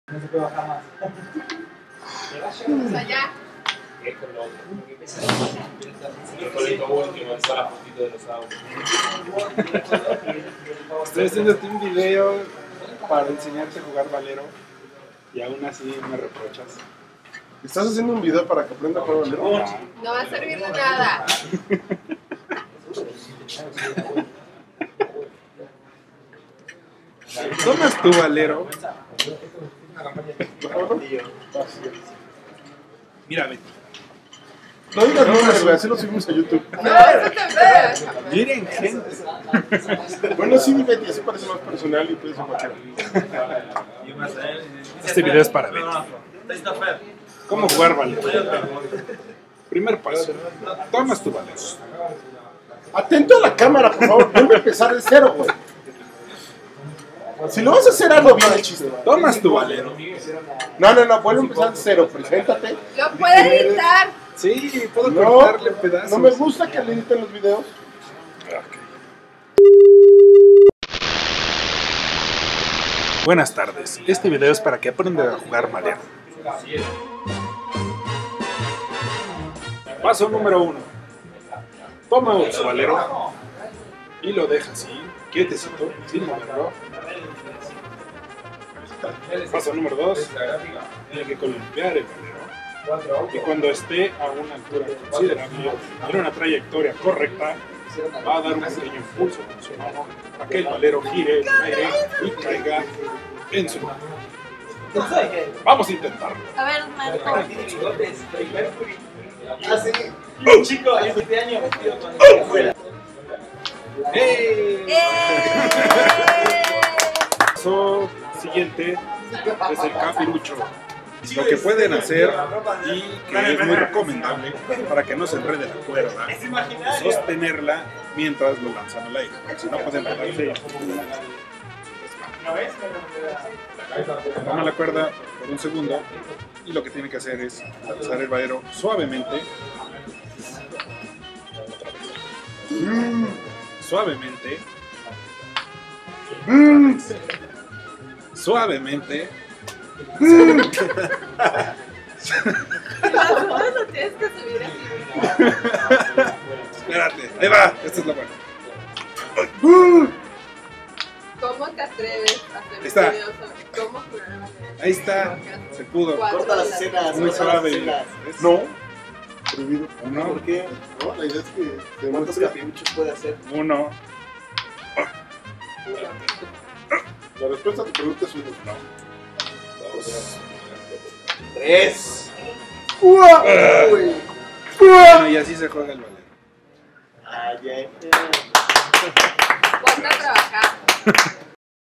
Cómo jugar Balero
A pesar de sus numerosos intentos, a Bety simplemente no le es posible jugar balero, es por ello que me di a la tarea de crear este tutorial donde de una forma divertida comprenderan la ciencia detrás de un simple juguete logrando realizar un mínimo de 3 capiruchos... y bajar 10 kilos.